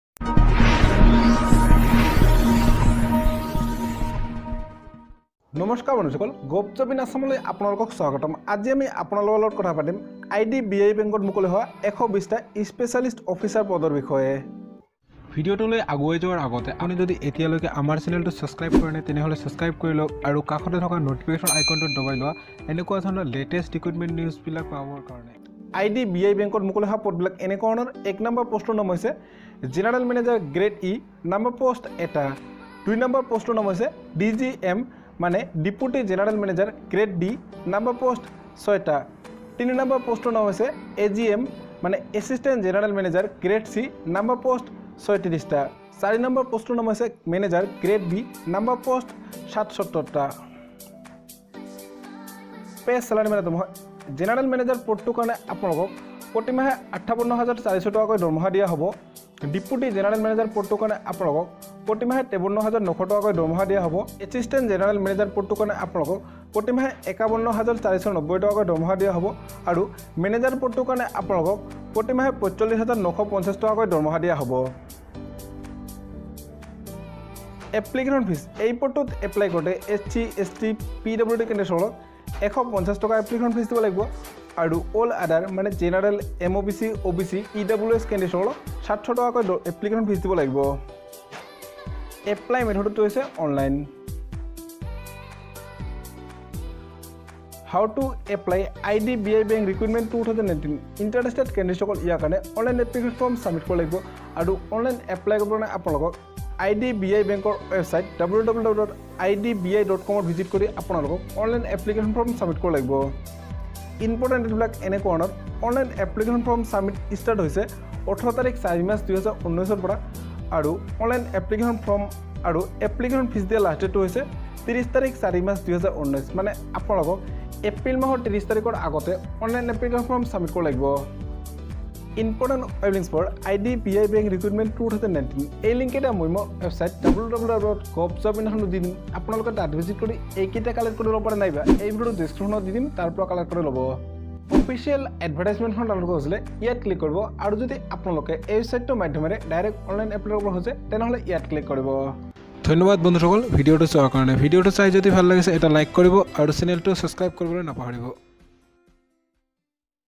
Bank job 2019 | 120 vacancy private job | Last Date - 30.04.2019
IDBI Bank recently published a job news for Specialist Officers in various 120 position. to online application submission for the following job vacancies in IBDI Bank.

◆    Last Date to Apply Online & Payment of Fee: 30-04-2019